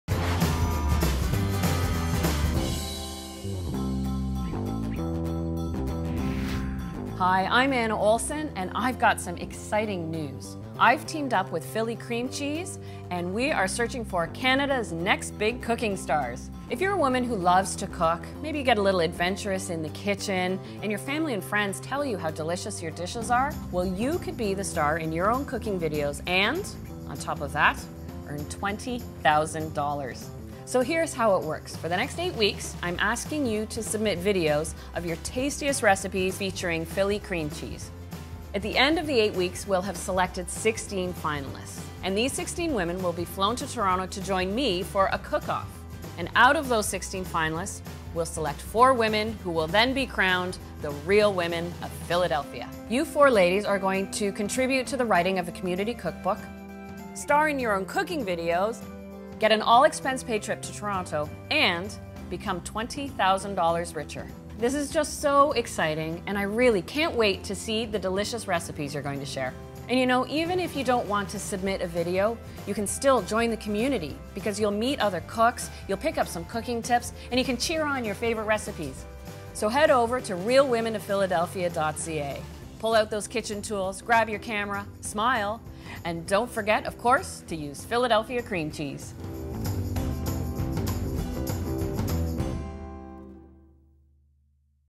About: Real Women of Philadelphia Canada
Anna Olson will be your host as we search for 4 of the next great cooking stars to be crowned the Real Women of Philadelphia . Together, the 4 winners will help lead the site, host their own cooking videos, help contribute to a community cookbook of Philly inspired recipes and have a chance to take home $20,000 each.